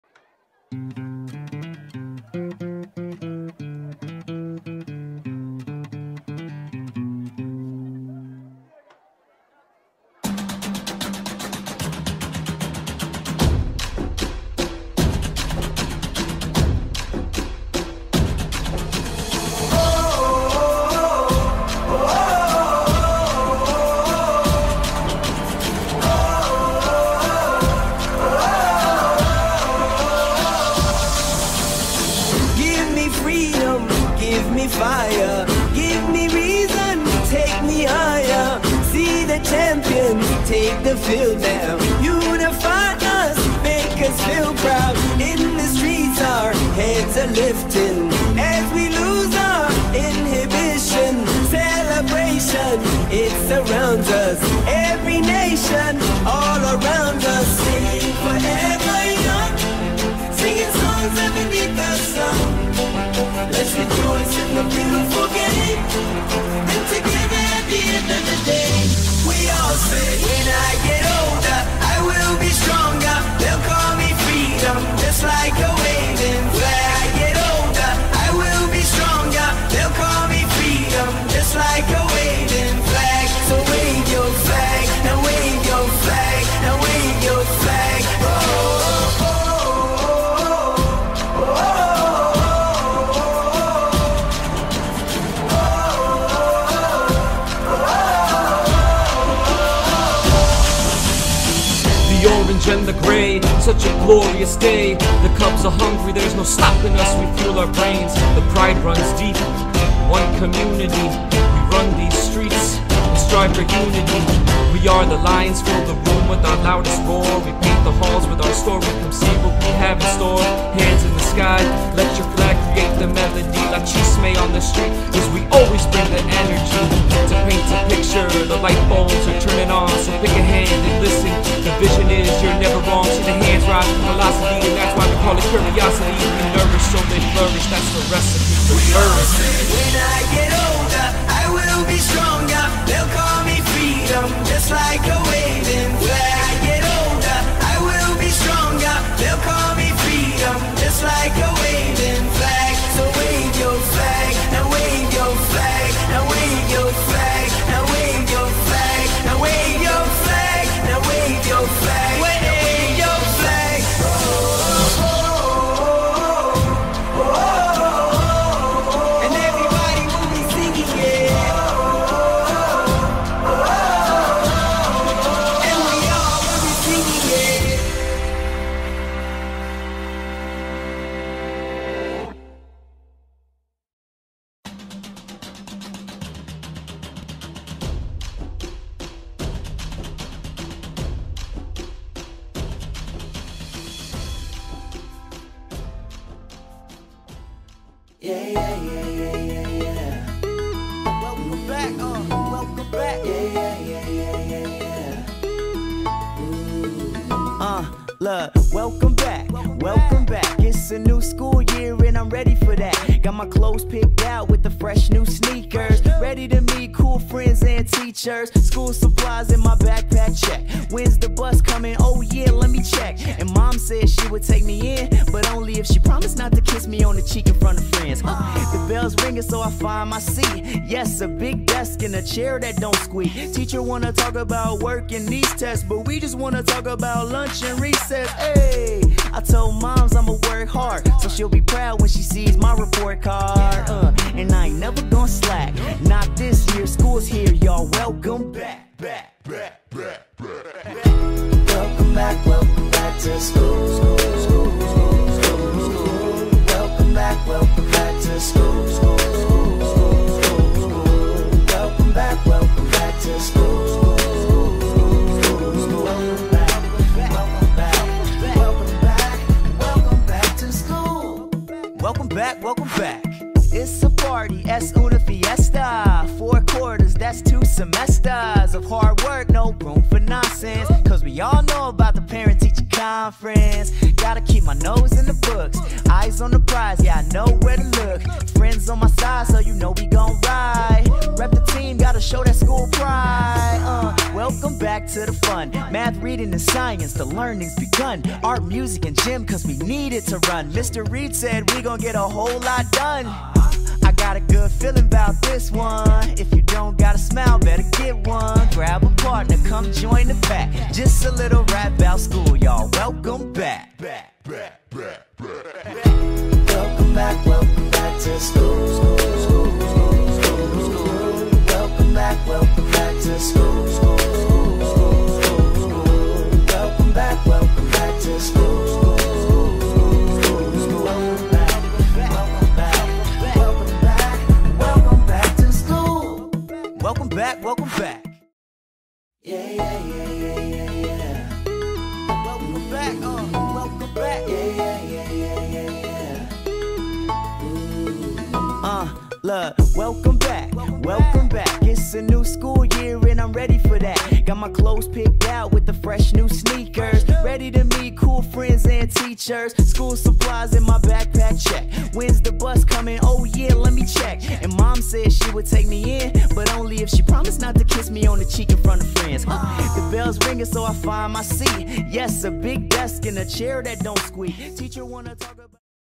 East Elmhurst Community School Student Welcome
Annual celebration welcoming students back to the proudest school in Queens!